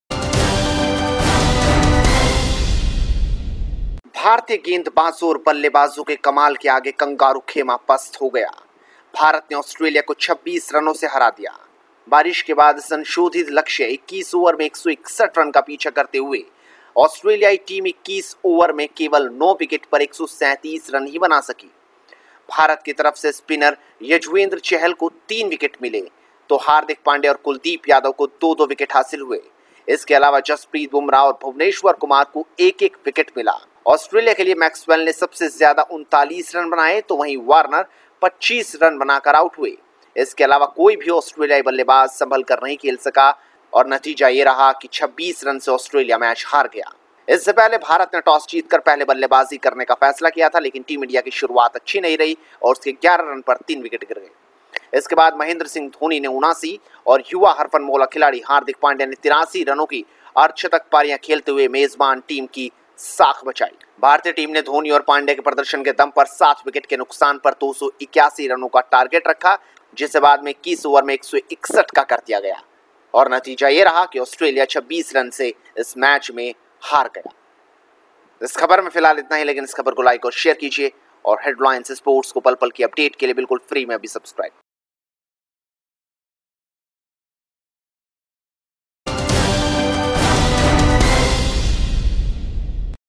Highlights: India won by 26 runs in 1st ODI against Australia in Chennai | Headlines India
India won by 26 runs in 1st ODI against Australia in Chennai.